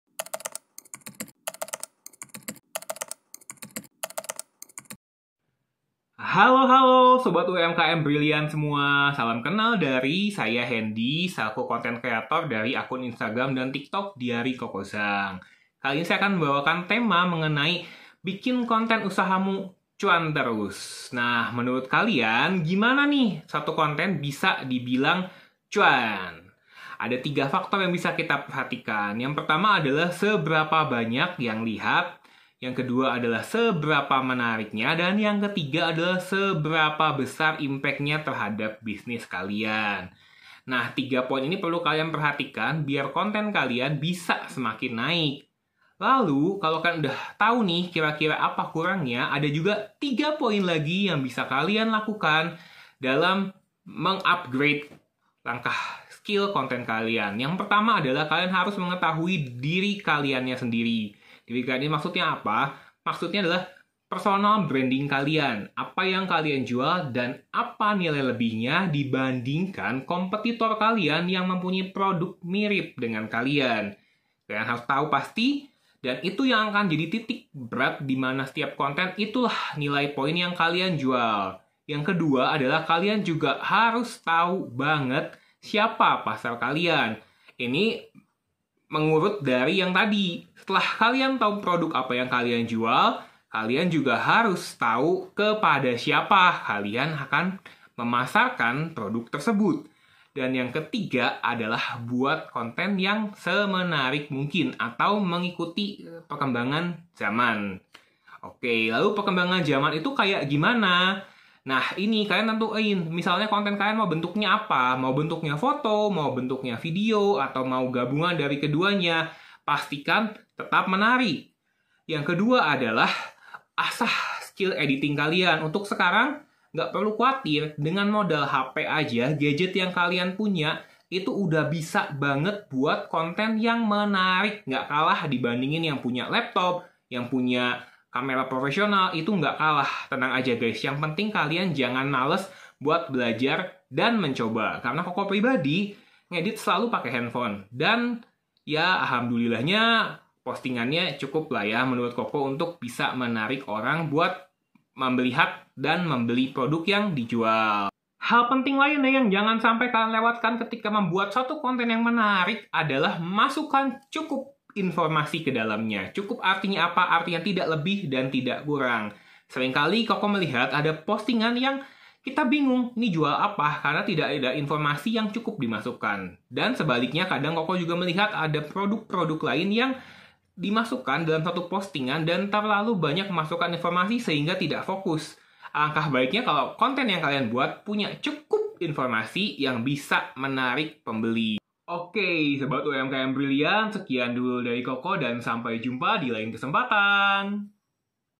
Strategi Bikin Konten Usahamu Cuan Terus
Yuk simak penjelasan Kokoh Zhang tentang strategi bikin konten usahamu cuan terus